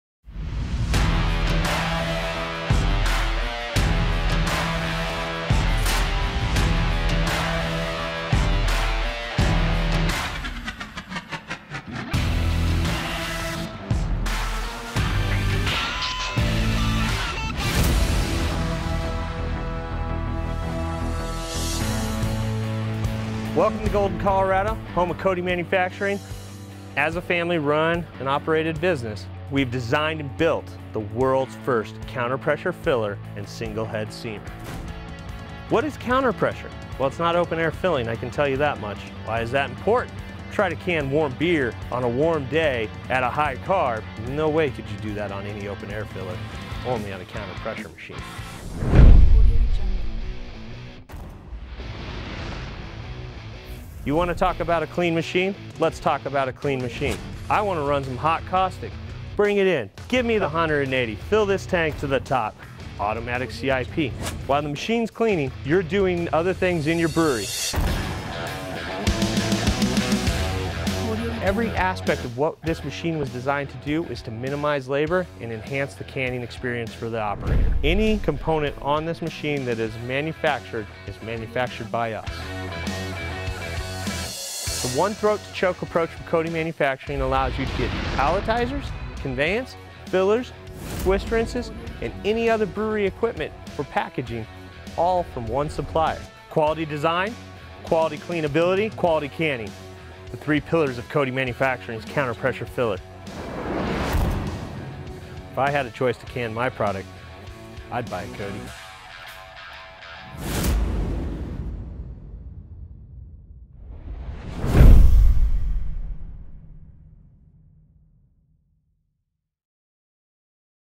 CODI Counter Pressure Canning System
See how the CODI canning system is more efficient and easier to maintain than other options on the market.  Counter pressure technology minimizes DO levels while allowing for a wider range of temperatures and carbonation levels when filling. The system can be expanded with depalletizers, twist rinses, conveying, pasteurizers and a variety of other options to automate the packaging process. Once the system is dialed in, you will spend less time babysitting your machine and more time creating beverages people will love.